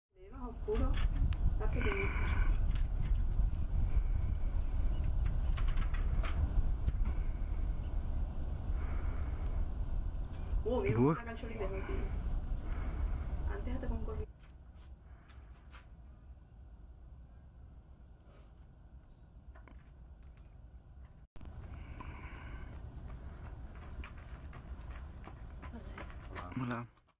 Broad-billed Sandpiper in the Bay of Cadiz
On the 7th of August 2018, Fernando Gross, a young birder, found this Broad-billed Sandpiper at Salina Carboneros, within the Bay of Cadiz Natural Park. It was relocated the day after by Fernando himself and Rafa García Costales. We were there shortly afterwards! Great views of a rare bird for the province of Cadiz and overall Spain indeed!